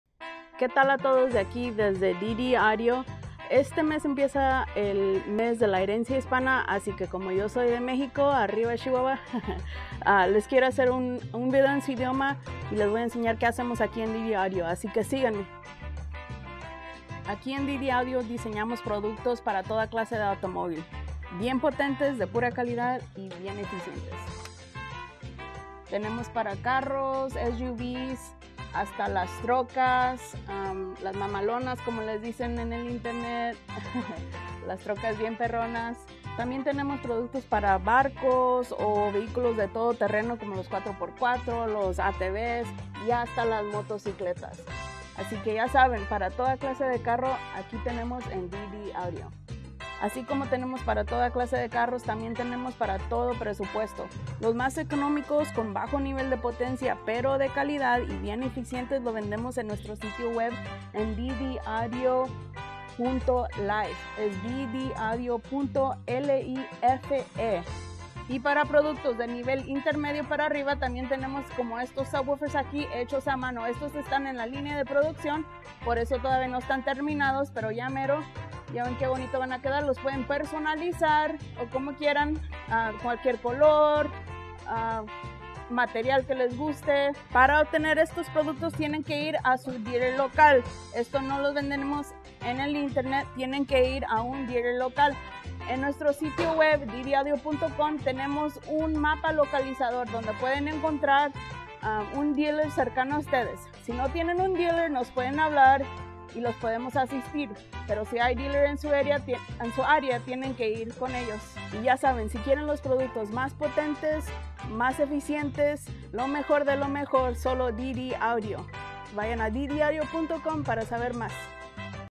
Hello (Hola) To Our Spanish Speaking Fans!
👋 ¡Hola! Un saludo muy grande a nuestros fans de habla Hispana!

Don't adjust your settings! We're sending a big hello to our Spanish speaking fans.